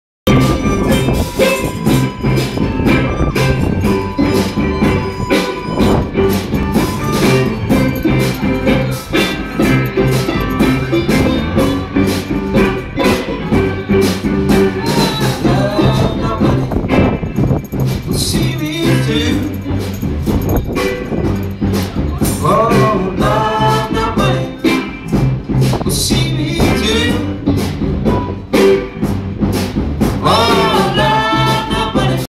06 07 2008 Band Broadstairs Pier
A Deal band busking at Broadstairs pier, have no idea who the band members are or even the name of the band, but still enjoyable